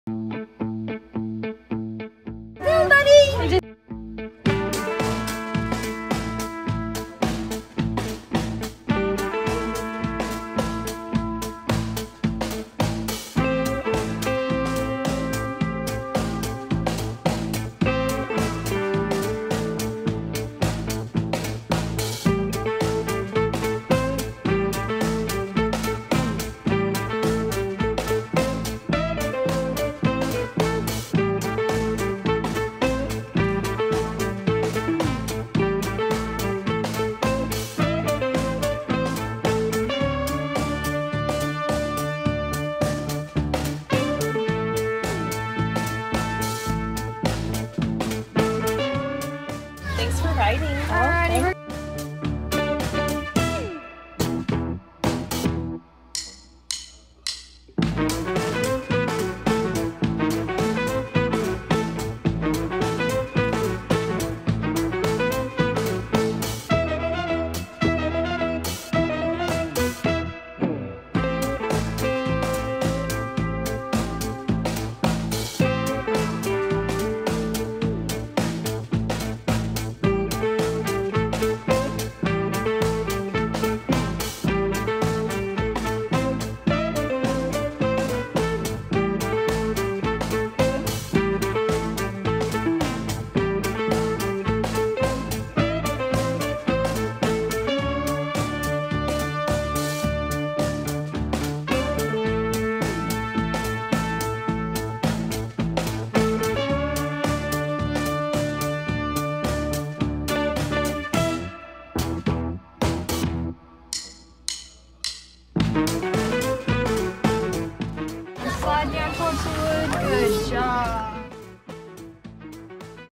Addie & Kaysen go camel riding at the Louisville Zoo 🐪
Addie & Kaysen camel 🐫 riding at the Louisville Zoo